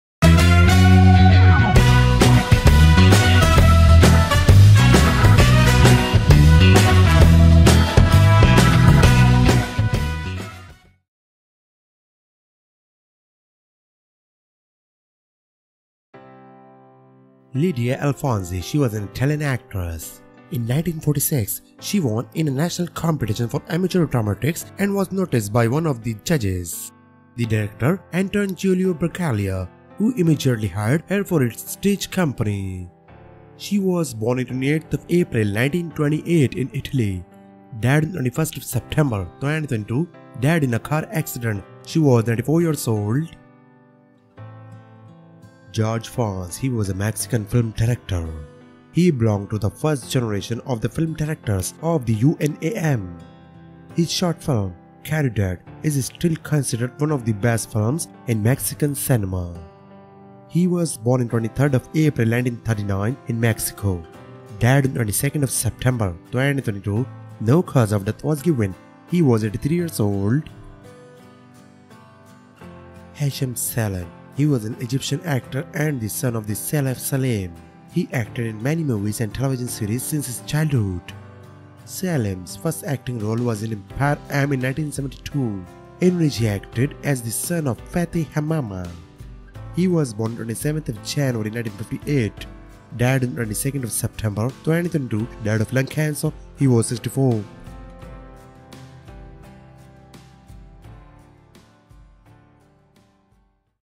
Actors Died Today 22nd September 2022
Actors died today on 22 Sep 2022.

List of Actors Who Died Today (ActorsDiedToday)
Lydia Alfonsi
Hesham Selim
Jorge Fons